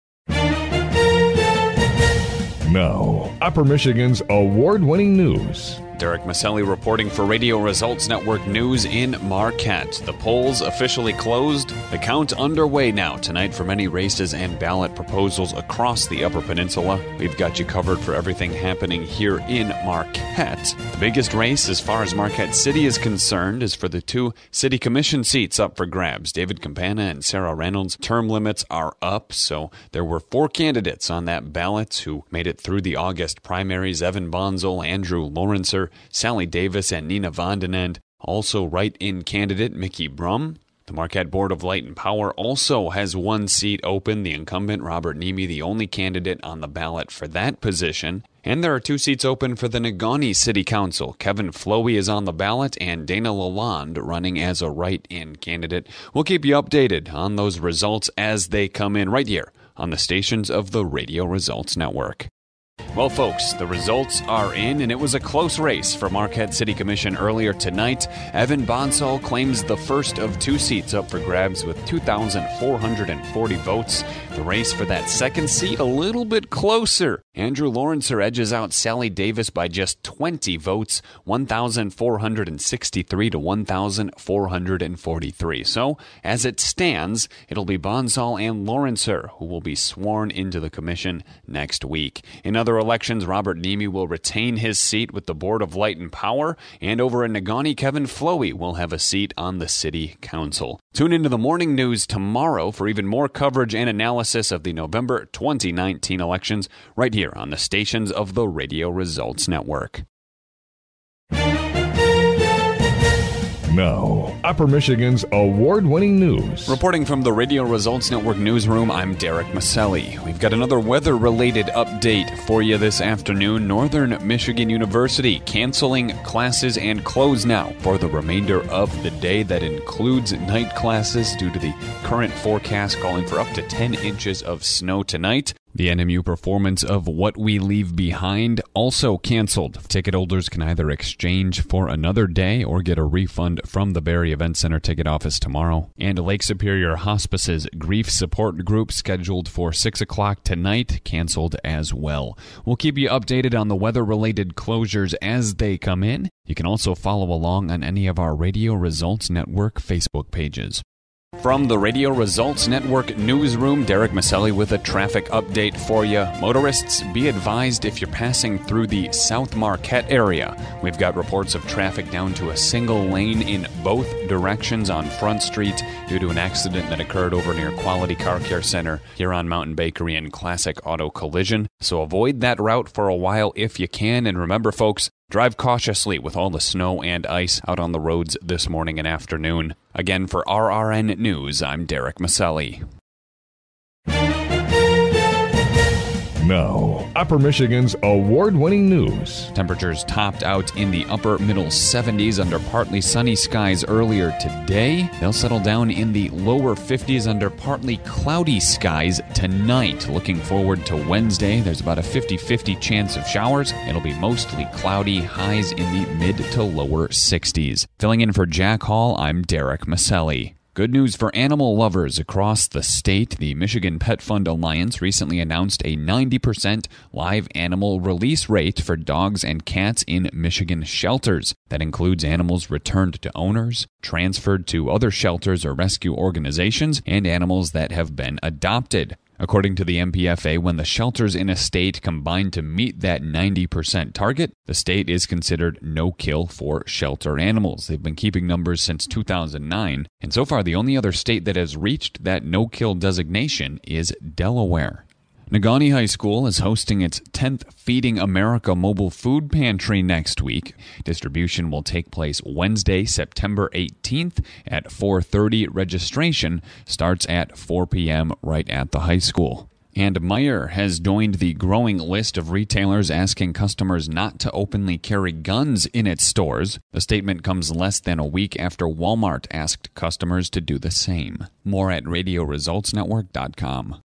News, Weather, and Traffic Reports
Demo for Derek Miceli consisting of various news/weather updates. More available upon request.